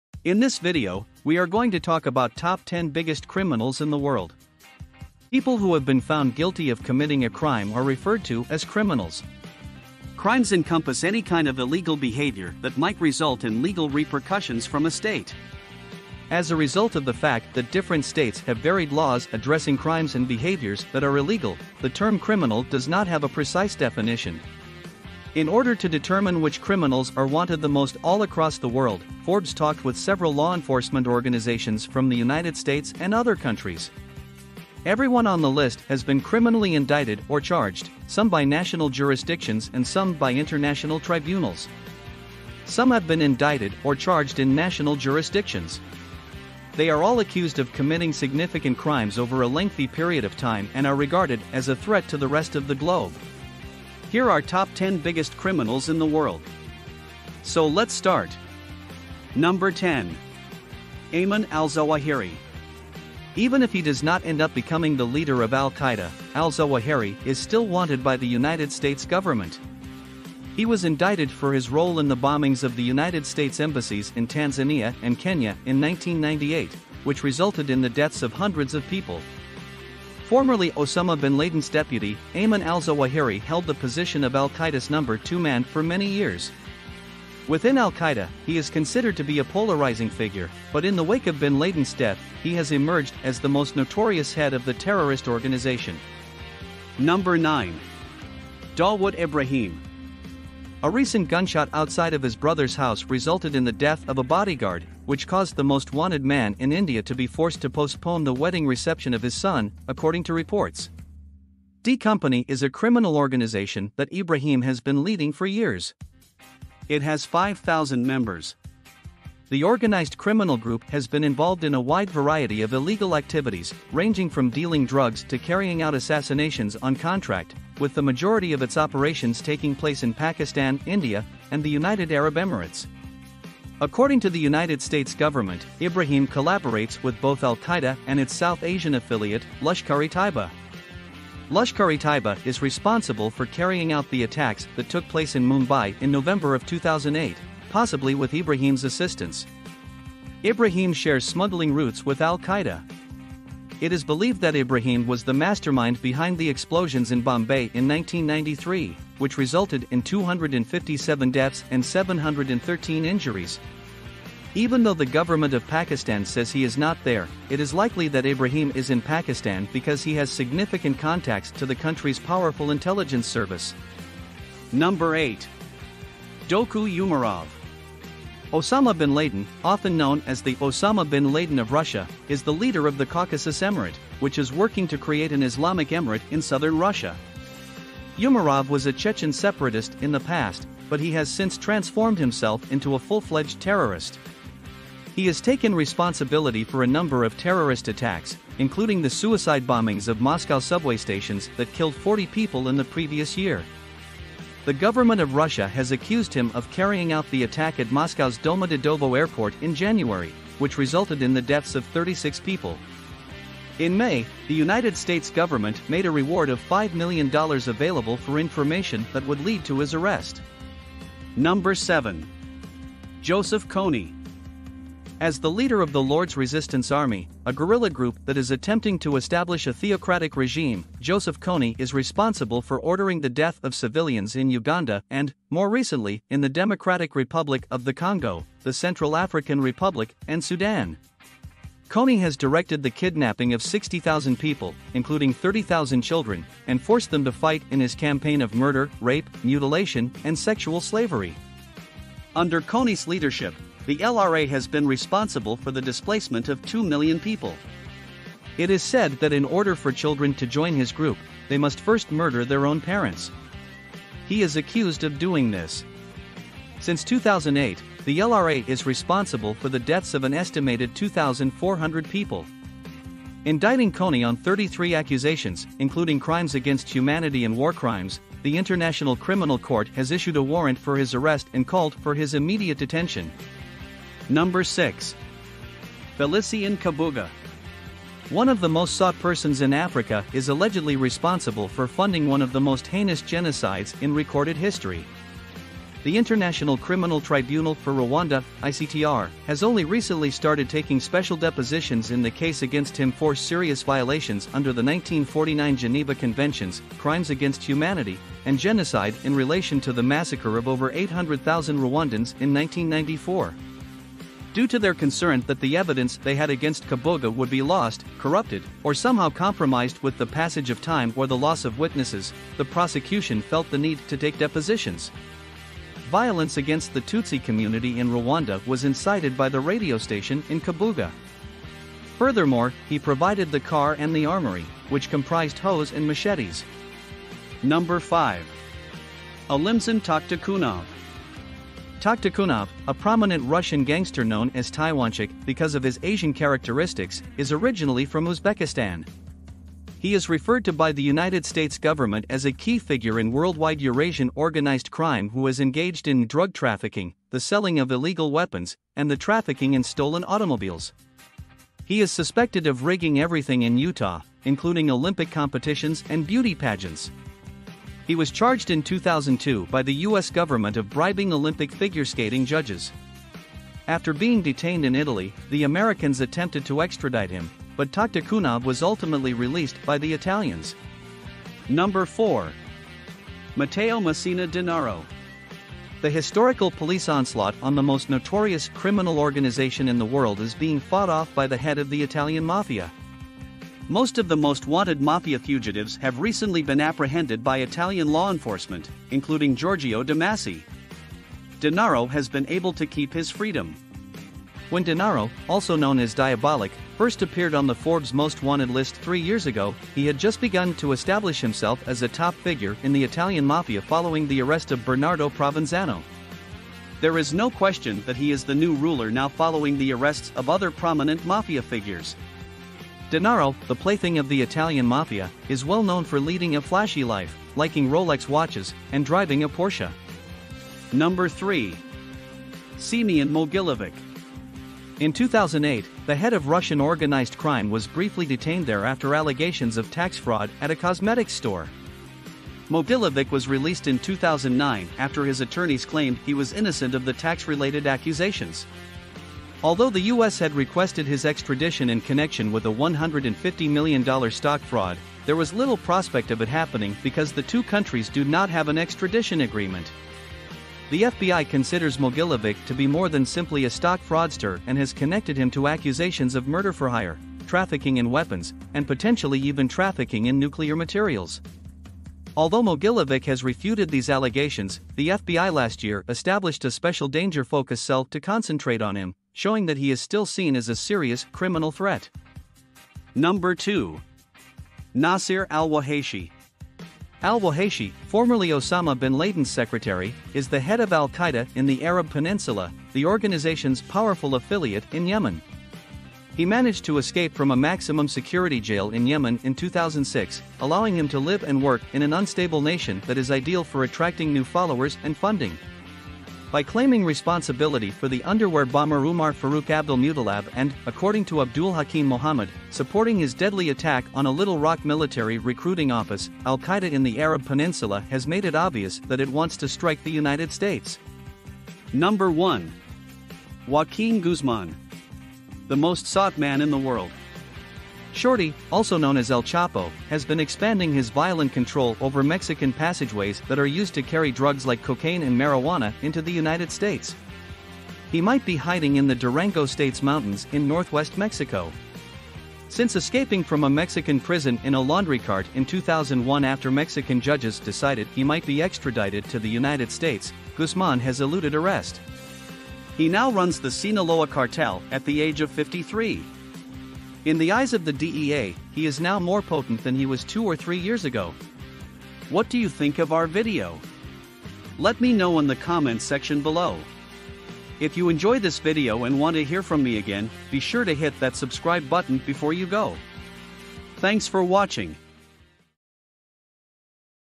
The 10 Most Dangerous Criminals in History
In this video, we are going to talk about top 10 biggest criminals in the world.

People who have been found guilty of committing a crime are referred to as criminals. Crimes encompass any kind of illegal behaviour that might result in legal repercussions from a state. As a result of the fact that different states have varied laws addressing crimes and behaviours that are illegal, the term "criminal" does not have a precise definition.

In order to determine which criminals are wanted the most all across the world, Forbes talked with several law enforcement organisations from the United States and other countries. Everyone on the list has been criminally indicted or charged, some by national jurisdictions and some by international tribunals. Some have been indicted or charged in national jurisdictions. They are all accused of committing significant crimes over a lengthy period of time and are regarded as a threat to the rest of the globe.

Discover the world's most interesting topics with Top 10! 🌍
✅ Get the latest news and updates on the most trending topics
🤩 Dive into the world of knowledge and entertainment